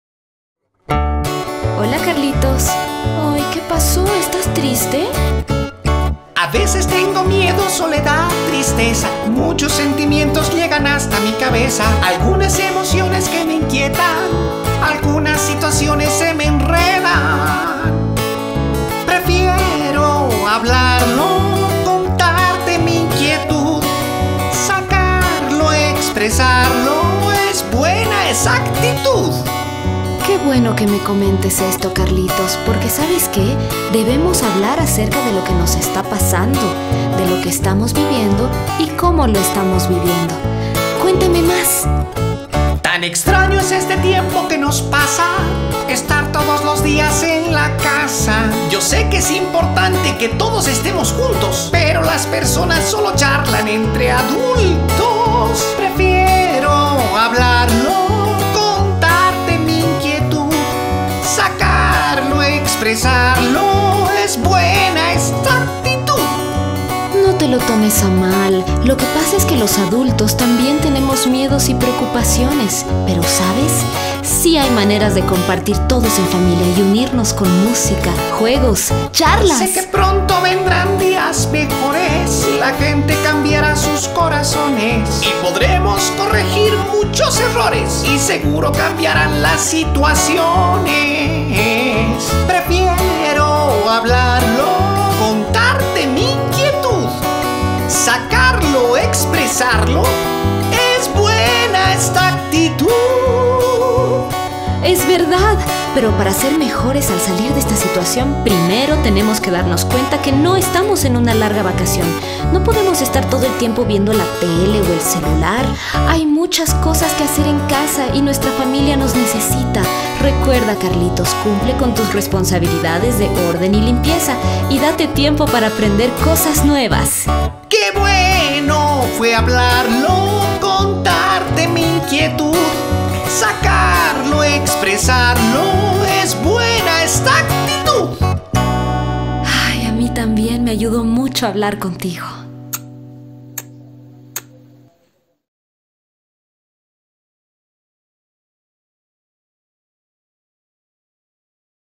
Qué le pasa a Carlitos
Tiempo de Estar Cerca es una iniciativa de Unicef Bolivia, la Mesa de Niñez del Pacto Global y la Confederación de Empresarios Privados de Bolivia para promover el acercamiento de las familias, el cuidado cariñoso y las prácticas de salud preventivas enfocadas al relacionamiento de los colaboradores de las empresas con sus hijos.

¡Para que cada familia aproveche el tiempo de estar cerca!

Parar y respirar
Madre: Glenda Rodríguez
Niño: Emiliano Muñoz Rodríguez y Sebastián Muñoz Rodríguez
Productora Audiovisual Arterias Urbanas y Chakana Teatro